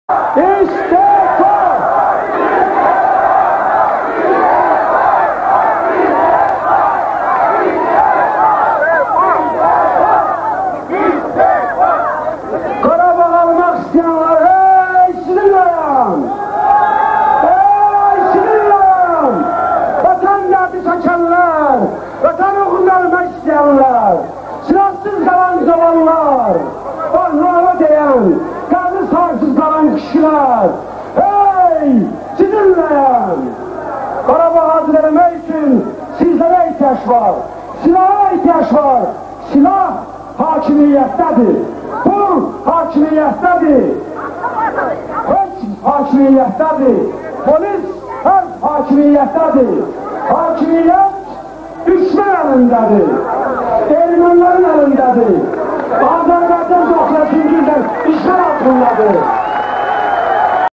Qurban Məmmədov: Hakimiyyət düşmən əlindədir! 13.04.2003 Mitinqİdən görüntülər.
Abunə olmağı və paylaşmağı unutmayın. Şərhlər bölməsində öz fikirlərinizi sərbəst ifadə edərək müzakirələrə qoşulun.